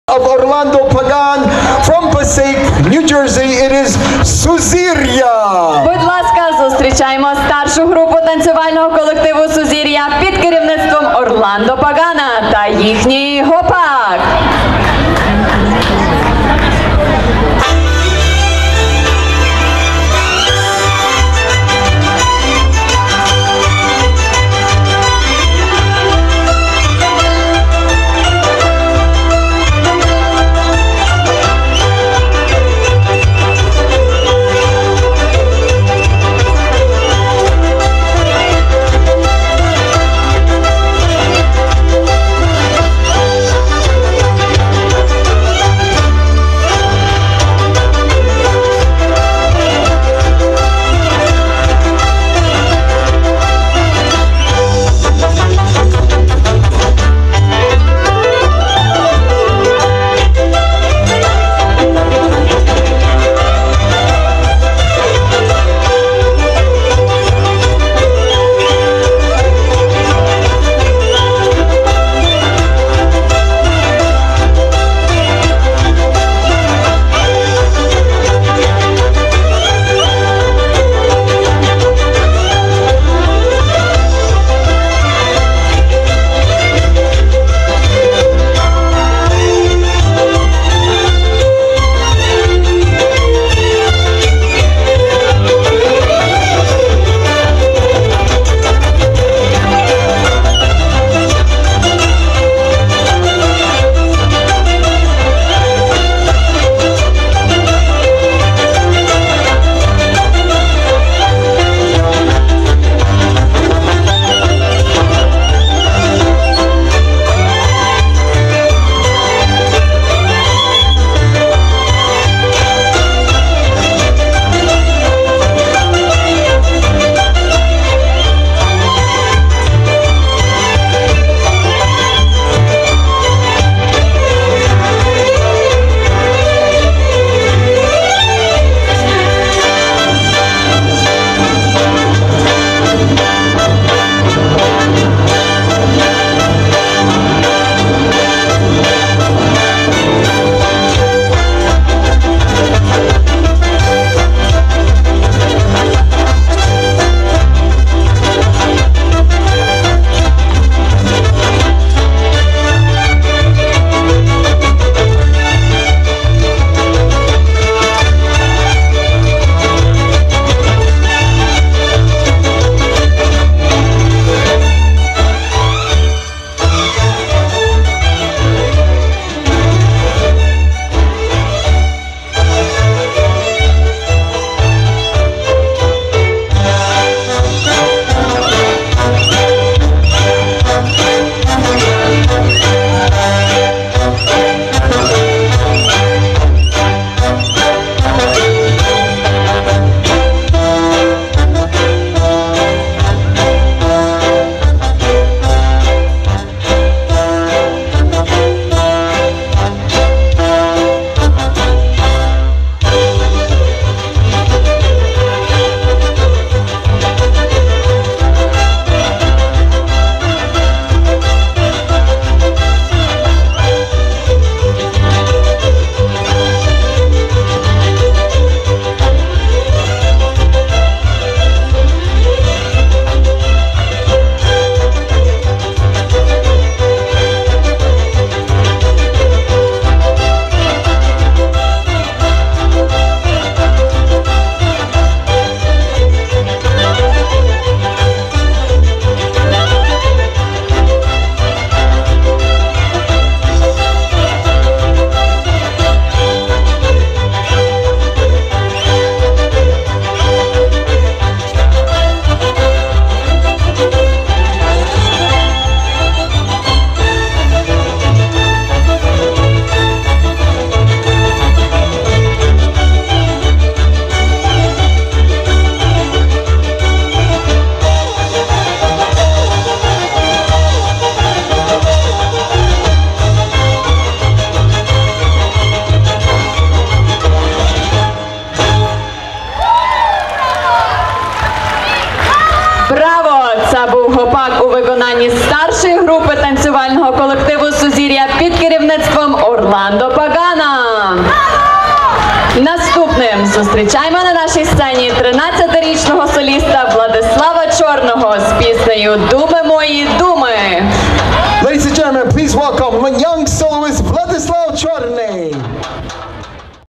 Suzirya Hopak NY 2016
Suzirya Hopak Dance under the direction of Orlando and Larisa Pagan Passaic 2016